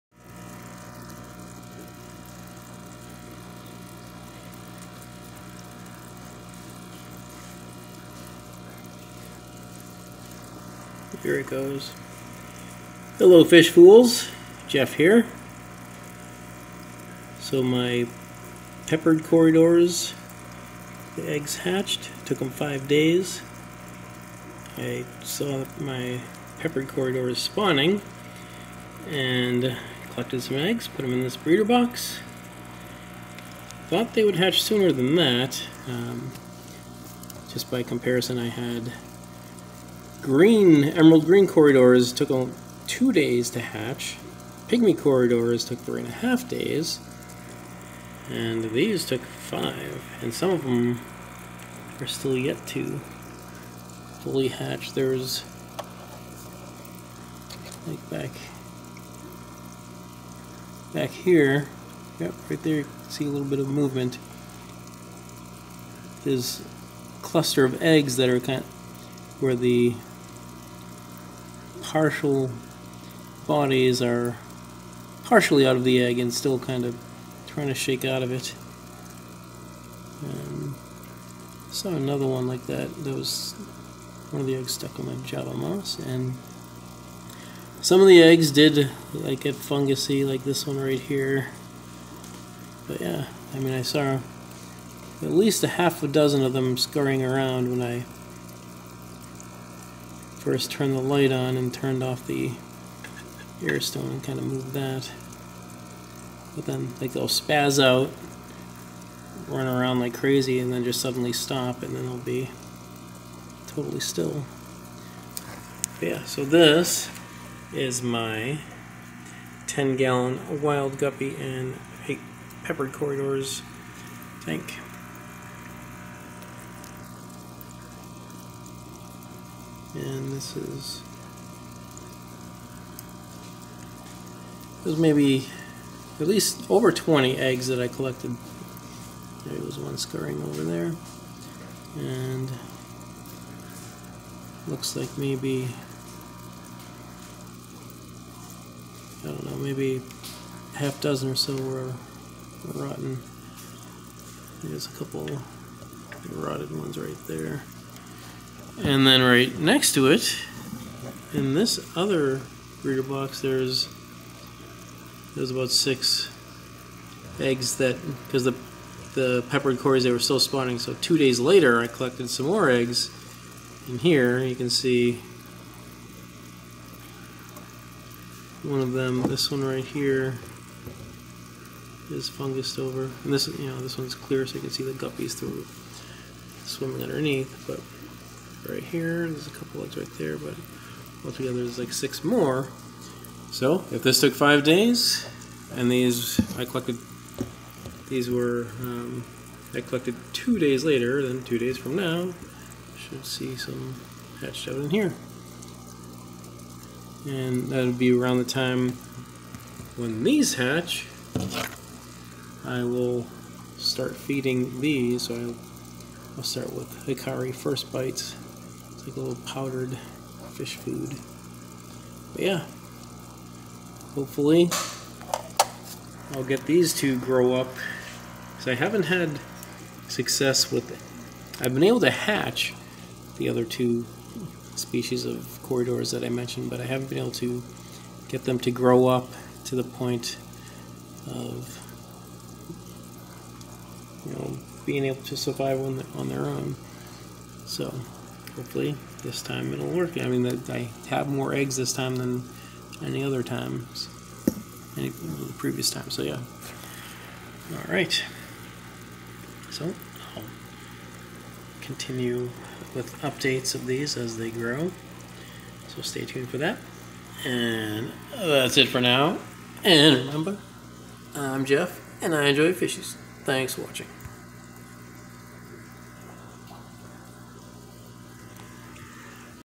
It Took 5 Days For Peppered Corydoras Eggs To Hatch
5 days ago I saw the peppered corydoras in my 10 gallon wild guppy tank. I collected over 20 eggs and put them in a breeder box and some have finally hatched out. There's at least a half dozen tiny little cory fry that I've seen scurrying around with more partially hatched.